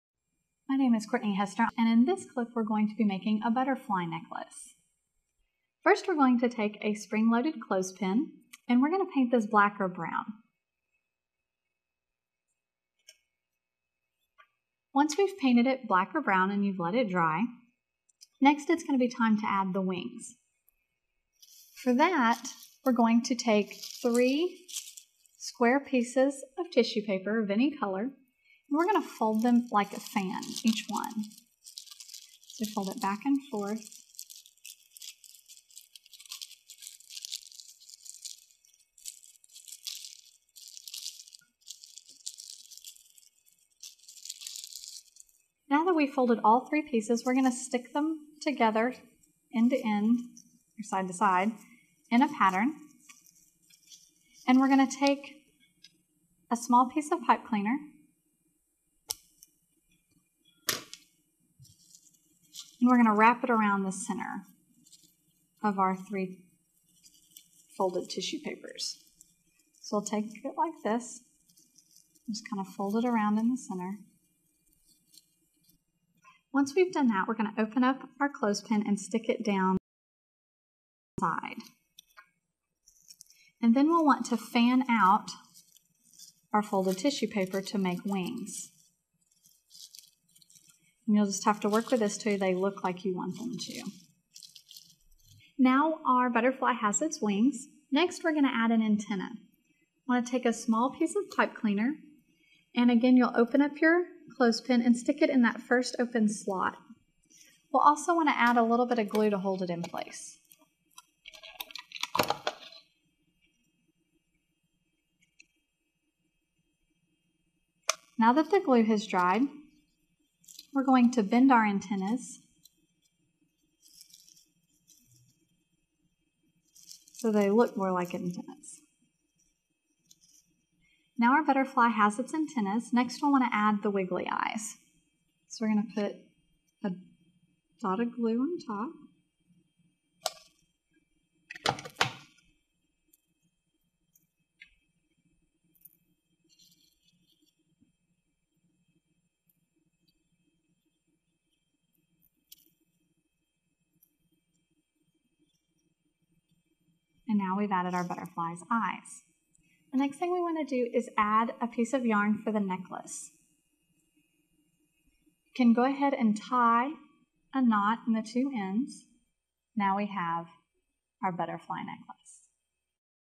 How to Make a Butterfly Necklace With Clothespins
How to Make a Butterfly Necklace With Clothespins. Part of the series: Arts & Crafts With Clothespins. Learn to create a butterfly necklace using clothespins, step-by-step instructions; learn this and more in this free instructional online arts and crafts video.